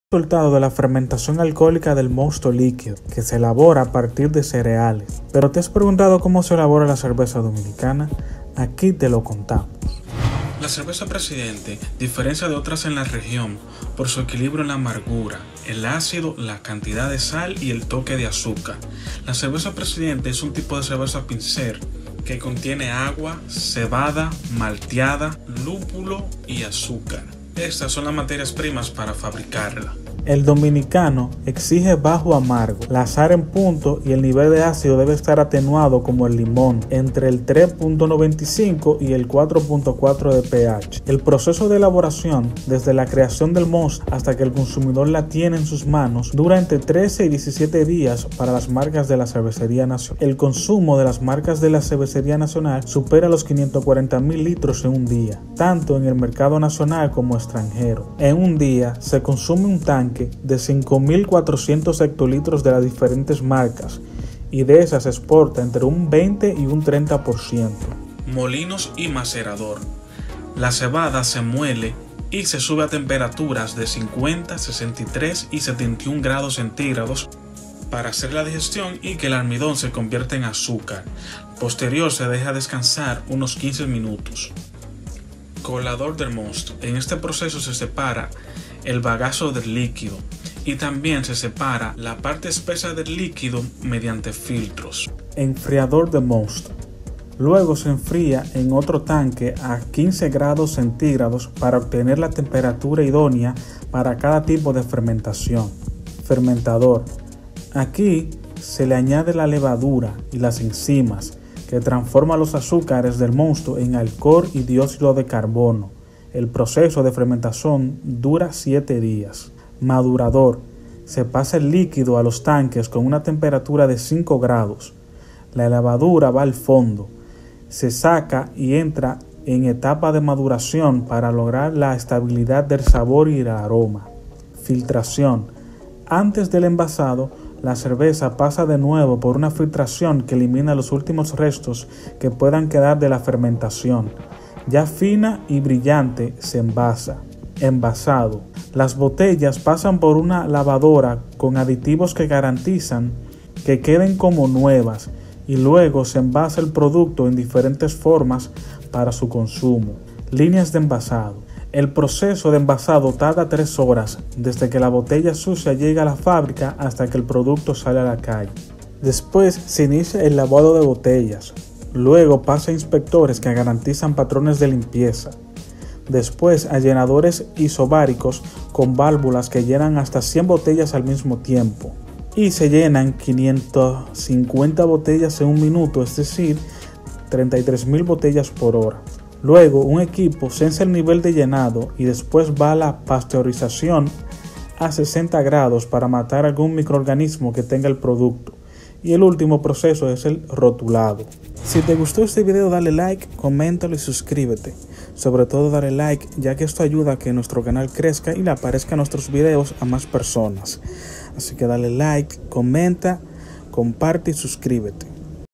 ASÍ SE HACE LA CERVEZA PRESIDENTE 🍻🍺
En este video te explicamos como se elabora la Cerveza Presidente, sus curiosidades, ingredientes y producto final.